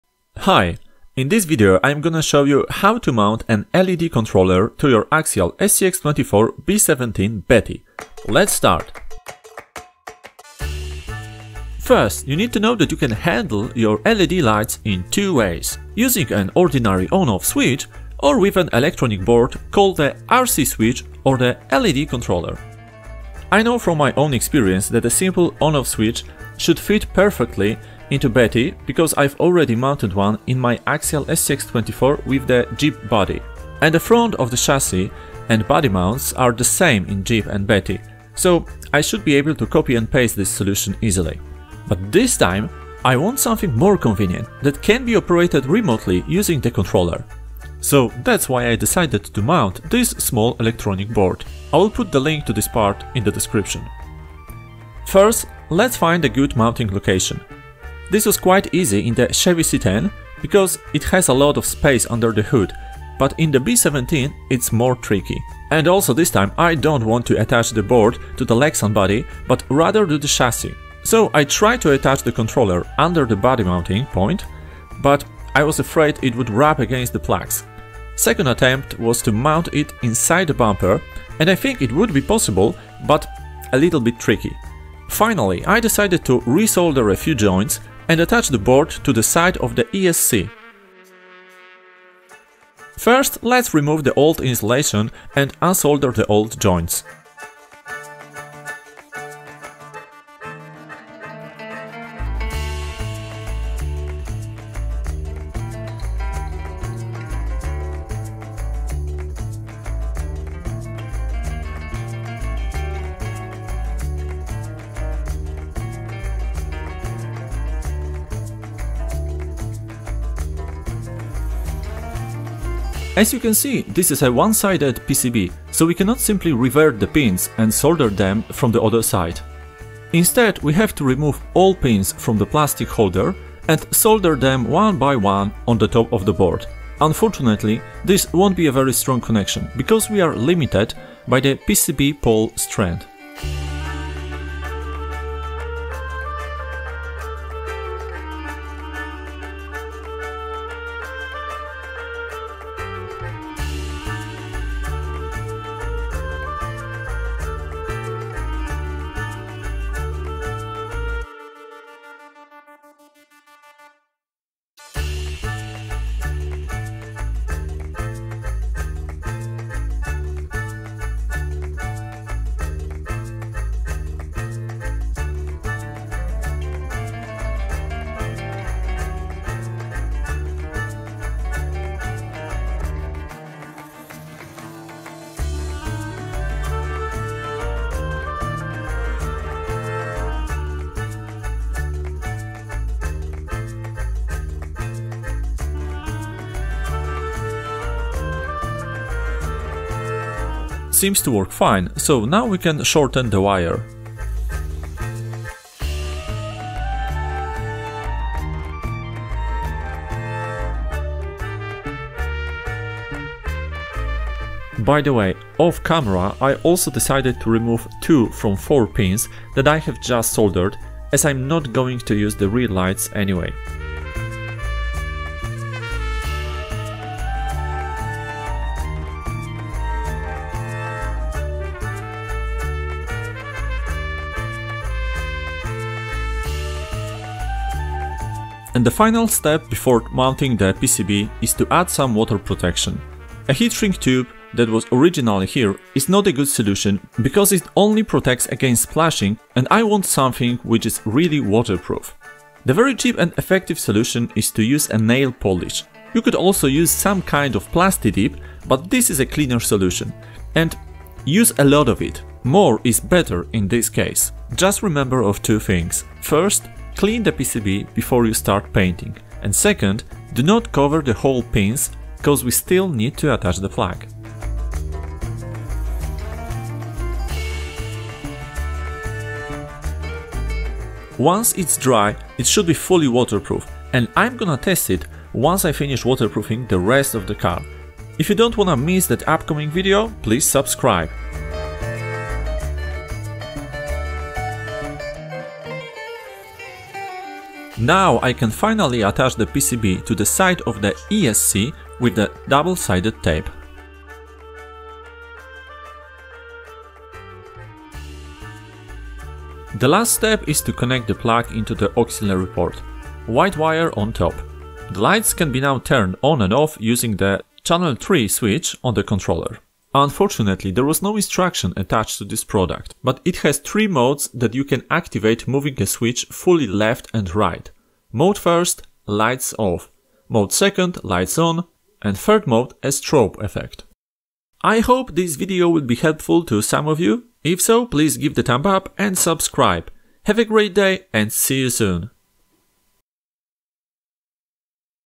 LED controller for the Axial SCX24 B-17 Betty
In this video I will show you how to remotely control the LED lights in the Axial SCX24 Betty but you can adapt this solution to any other RC car.

LED controller: